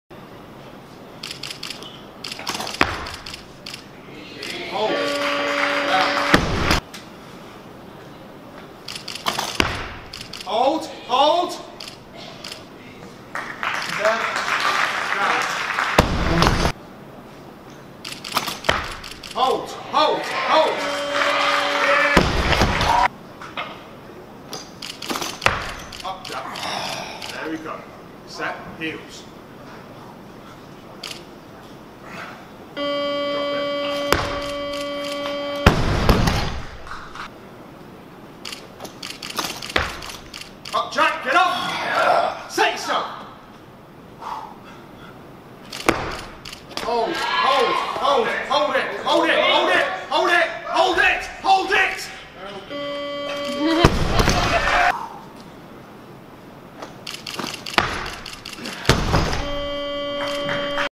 Jack Dobson 7th place at English Champs 2016
Jack Dobson (85) from Leeds City Weightlifting team goes 113/137 for a 250kg total at the English Champs, he finished in 7th overall in what I believe was his first ever national comp so well done Jack!